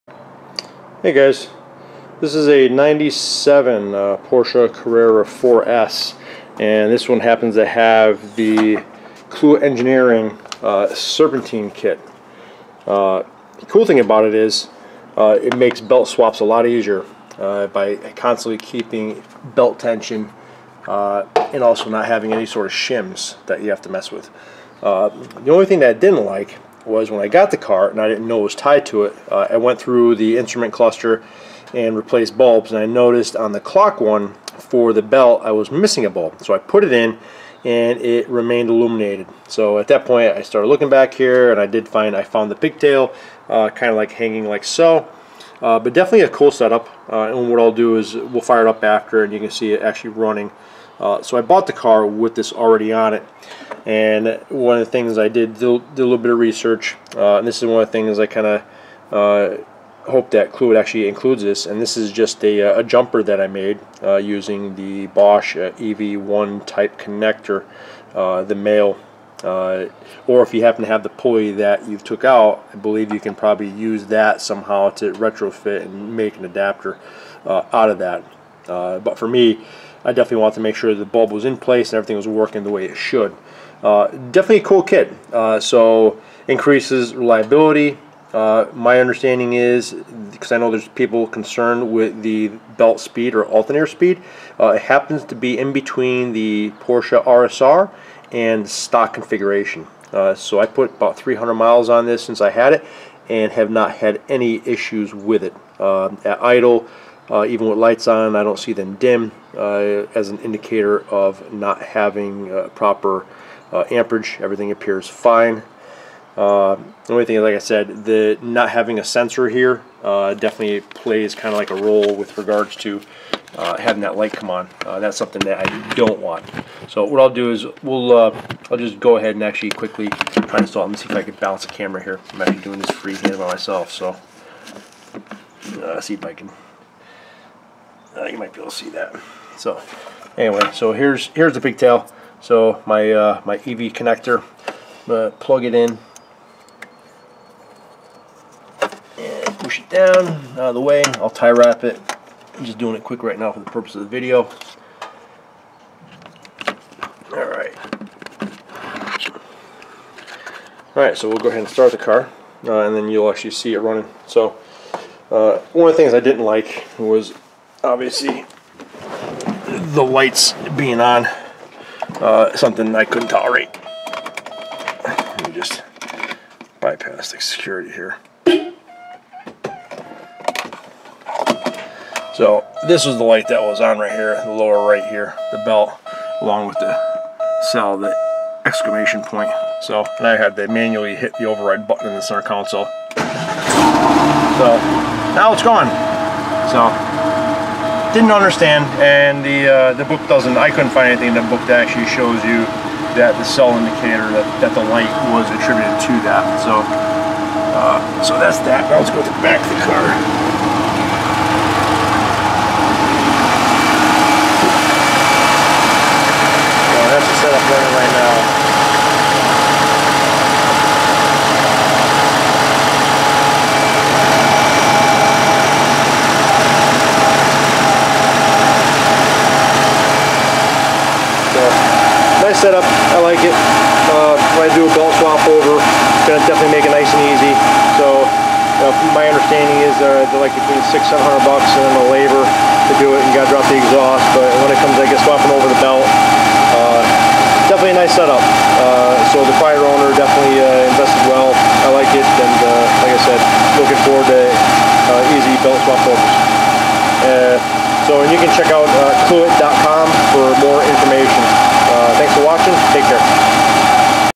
1997 Porsche 993 4S with Clewett Engineering Serpentine Belt Kit - Similar to RSR setup
Here we have a quick video of the Clewett Serpentine Belt kit on this 1997 Porsche 993 4S.  Nice and easy belt swaps - good by shims, reliable belt operation, and no need for a belt sensor since the 1 belt drives the alternator, if it breaks the light comes on.  for more info.

0:00 Start
0:54 Bosch EV1 Type Pigtail for obsolete Belt Tensioner
2:20 Pigtail install
3:04 Engine Start - showing clock lights
3:34 Engine running shot